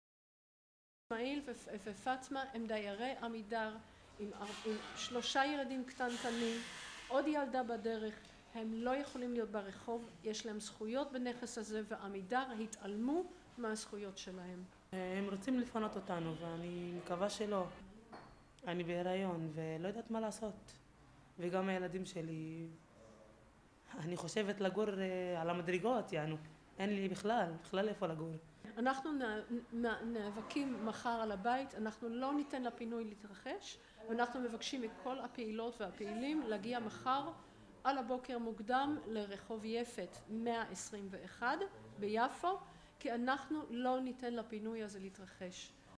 רגע לפני הפינוי מהדירה - מתבצרים
ביום חמישי (מחר 22/1/2015), מתכוונת המשטרה לפנות את משפחת אבו רחימה, זוג הורים ושלוש ילדות בנות שנה, שנתיים ושלוש, מדירת עמידר ברח' יפת 121 ביפו. פינוי לרחוב, בחורף. האישה בהריון בחודש שביעי. הבקשה לעיכוב הפינוי של משפחת אבו רחימה מיפו נדחתה. פעילות ופעילים נקראים לדירה ביום חמישי בבוקר, להתבצרות בה על מנת למנוע את הפינוי.